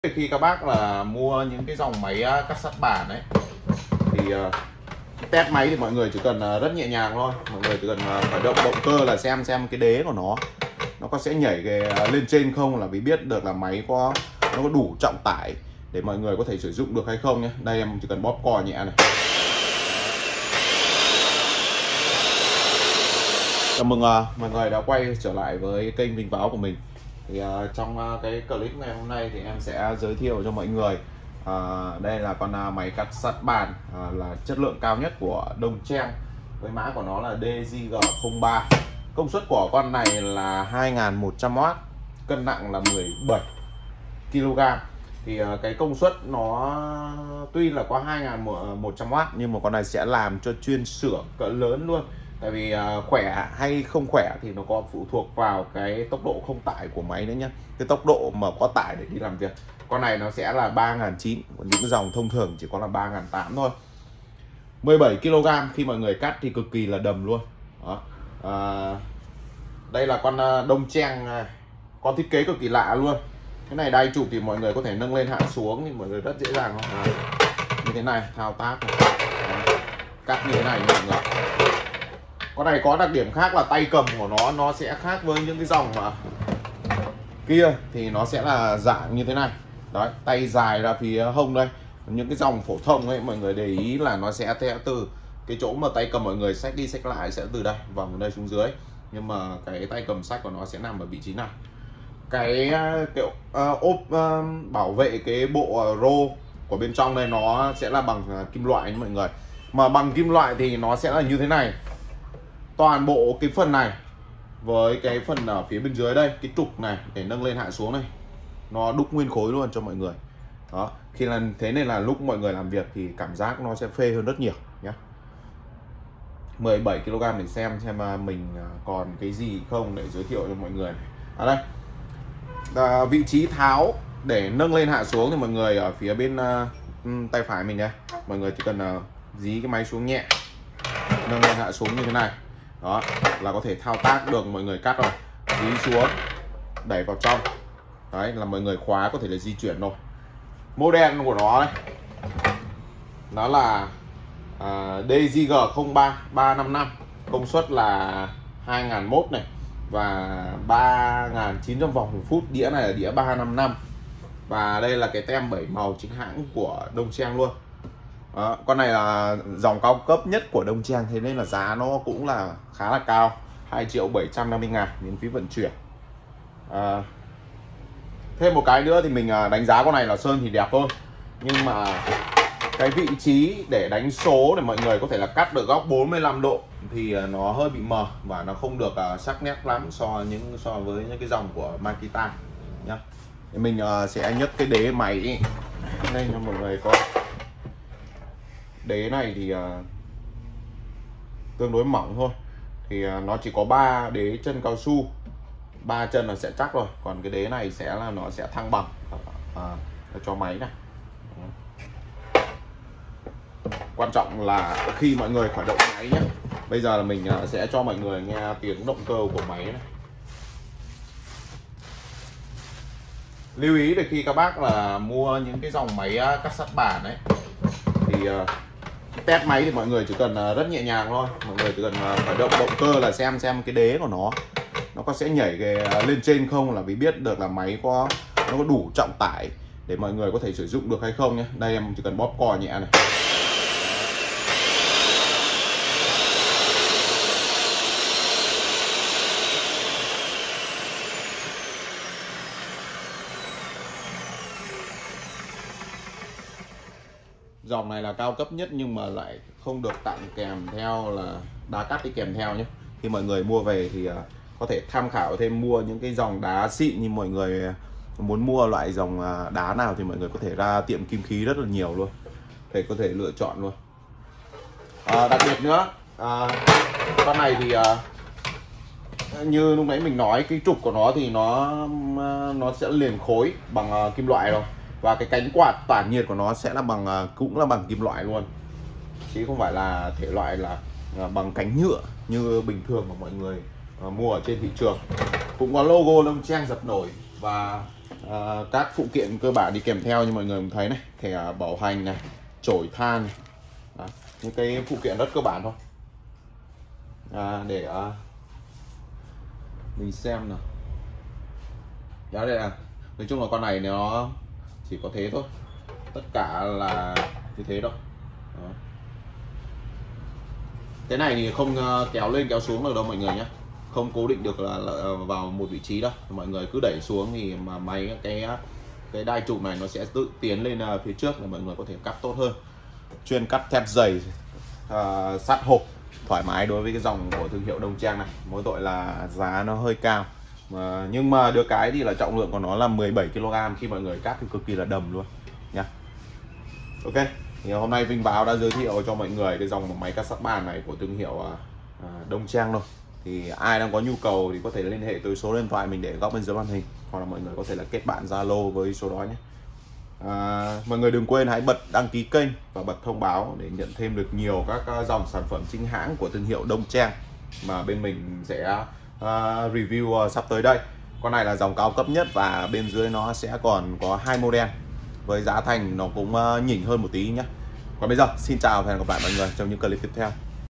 Máy Cắt Sắt Dongcheng DJG03 , Máy Cắt Sắt Bàn Trục Liền Khối | Vinh Váo
Giá :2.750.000 vnđ Miễn Phí Vận Chuyển

Thông Số Kĩ Thuật Máy Cắt Sắt Dongcheng DJG03

🔴 Hãng sản xuất : Dongcheng

🔴 Công nghệ :Trung Quốc

🔴 Công suất :2100W

🔴 Xuất xứ : Trung Quốc

🔴 Model :DJG03

🔴 Đường kính đá cắt 355mm

Máy Cắt Sắt Dongcheng DJG03 được sử dụng rộng rãi trong các công trình xây dựng , làng nghề

Máy Cắt Dongcheng  bao chụp thiết kế liền trục, chắc chắn , Cân nặng 17 kg Giúp cho máy cắt khả năng chính xác và đầm máy